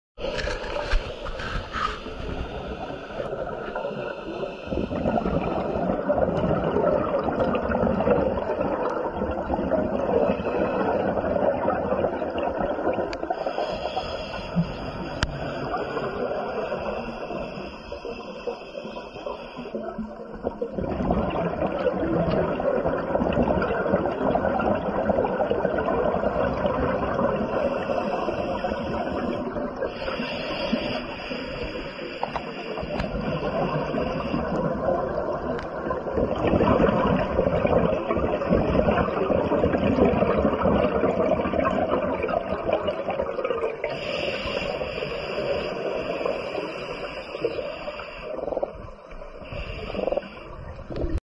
Scuba diving a Ww2 Shipwreck on Oahu Hawaii 2020 at 100 feet
Scuba diving a Ww2 Shipwreck dive on Oahu Hawaii 2020 at 100 feet